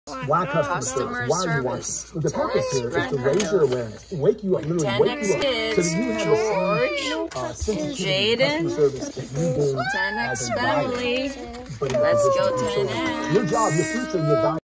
Day 158. Learning Cardone U from Grant Cardone w/ George & Jaden about Customer Service
1. Customer service is the process of ensuring a customer satisfaction with a product or service.
2.  Everyone has a responsibility to service a customer and deliver service beyond what they pay for.